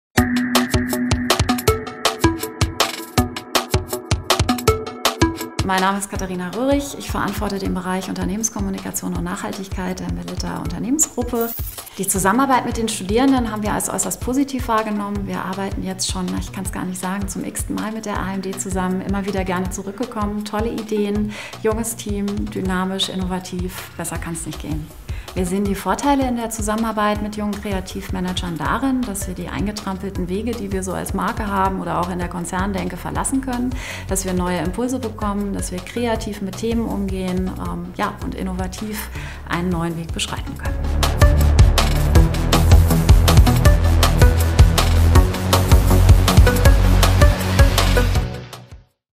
AMD - DESIGN AND INNOVATION MANAGEMENT (B.A.) | MELITTA - AMD Akademie Mode & Design
So studiert man heute: DESIGN AND INNOVATION MANAGEMENT (B.A.) an der AMD! Bewirb Dich jetzt!